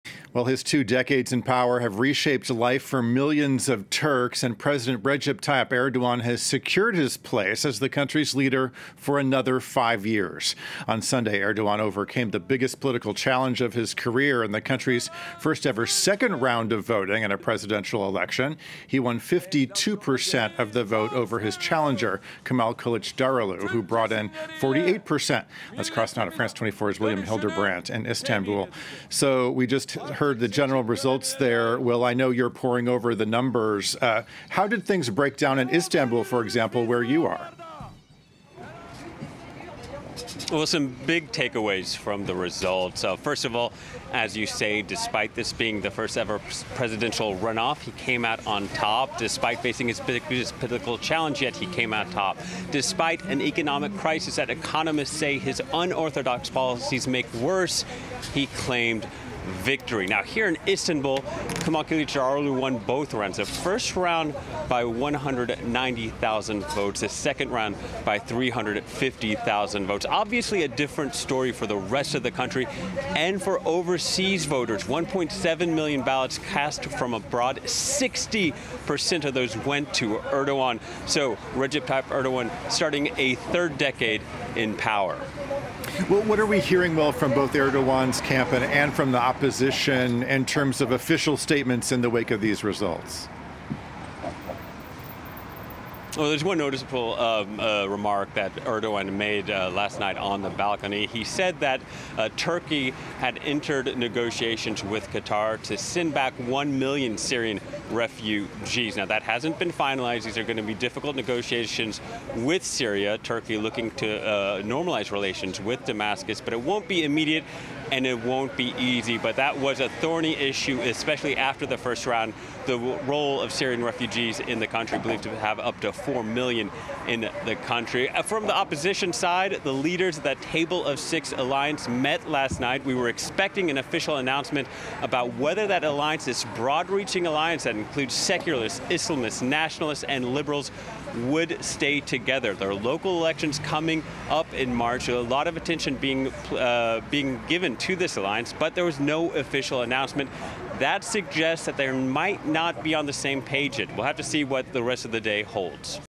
Turkey: Erdogan triumphs in election test, extending 20-year rule • FRANCE 24 English
The election had been seen as Erdogan's biggest political challenge, with the opposition confident of unseating him and reversing his policies after polls showed a cost-of-living crisis left him vulnerable.